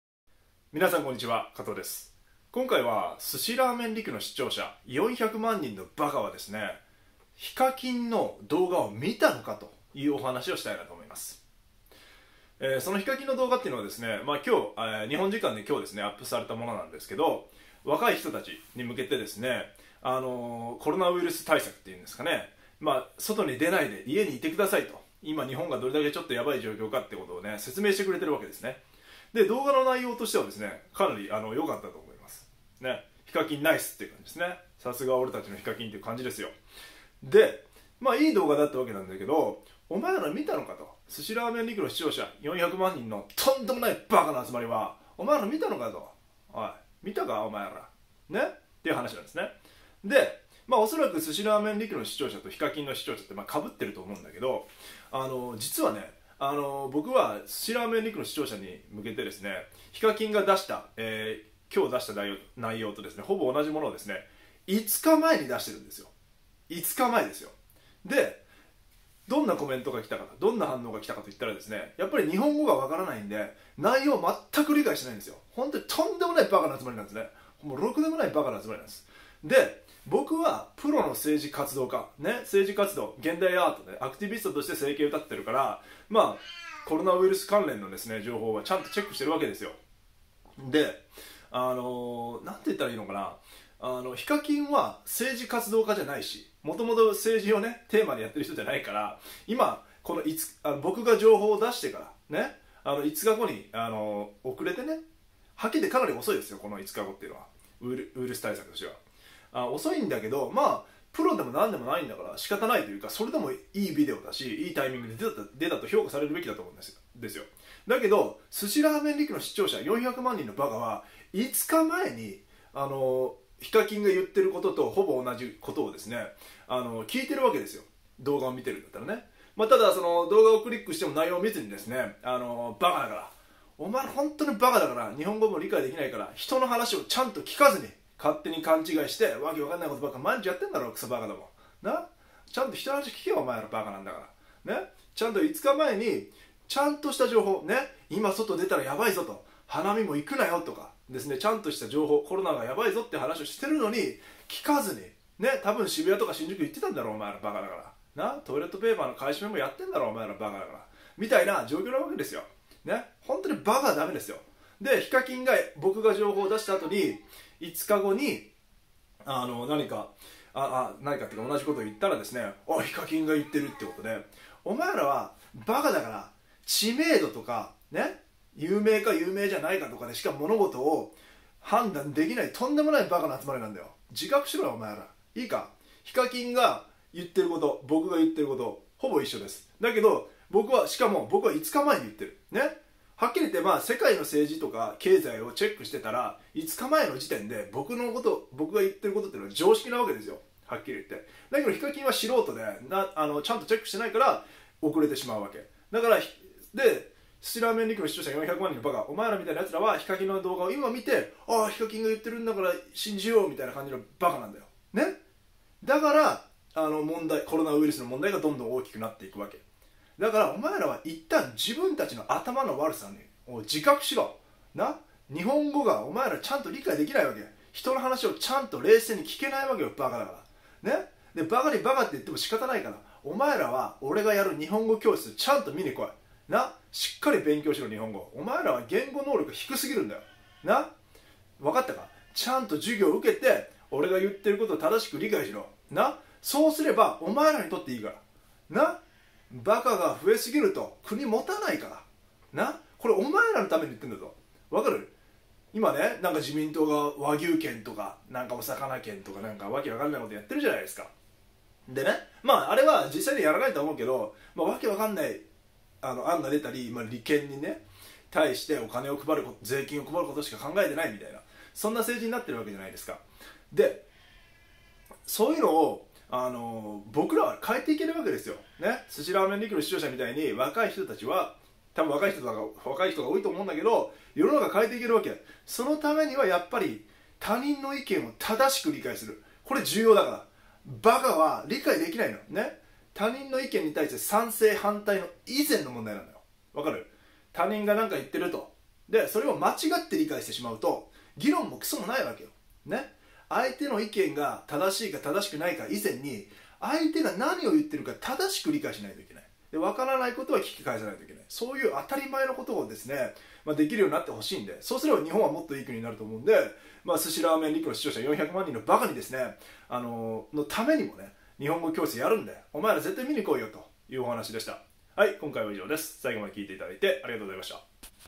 すしらーめんりくの視聴者400万人はヒカキンの動画見たか？
すしらーめんりくの視聴者400万人に説教します！！！